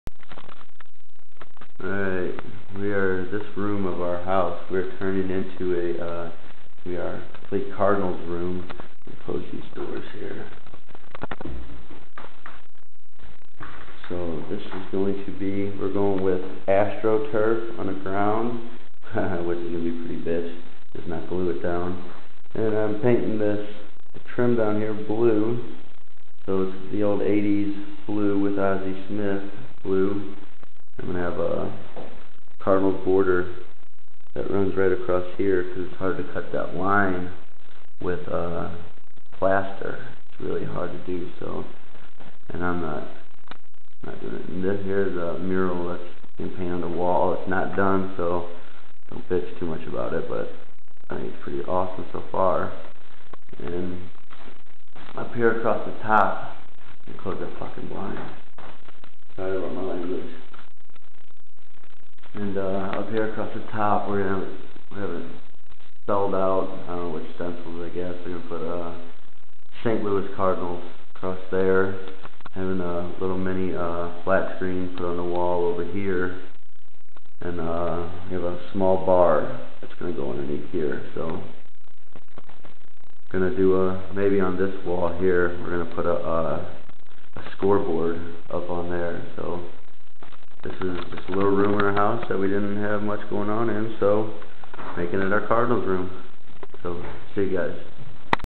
Starting on my Cardinals room bird is 1920's logo
We just bought a new to us house and it has a little room that we didn't know what do with The bird is the 1920's logo